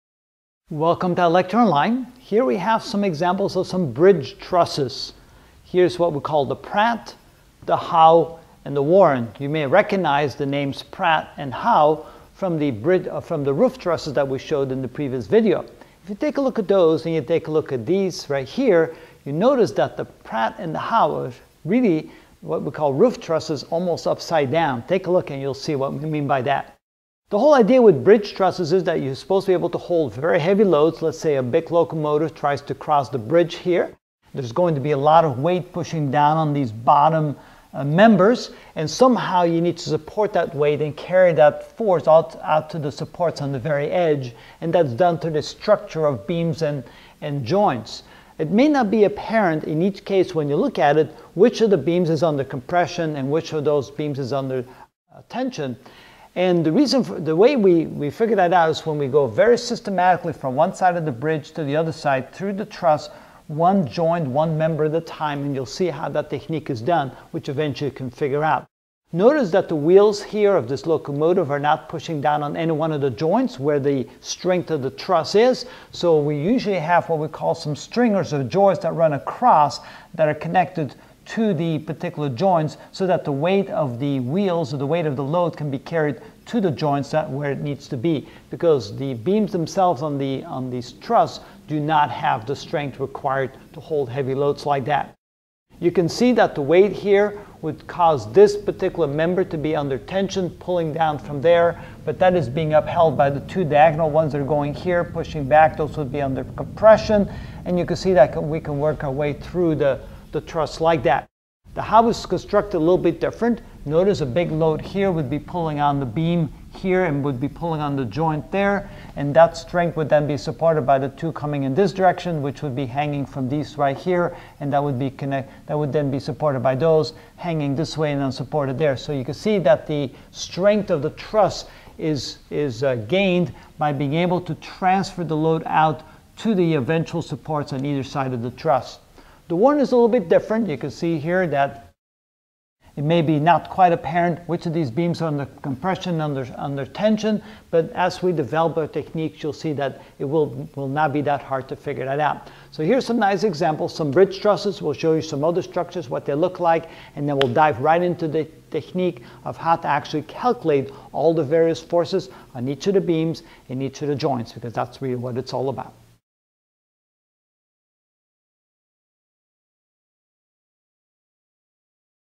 Mechanical Engineering: Trusses, Bridges & Other Structures (3 of 34) Ex. of Bridge Trusses: 1
In this video I will explain 3 examples the bridge trusses: pratt, howe, and warren.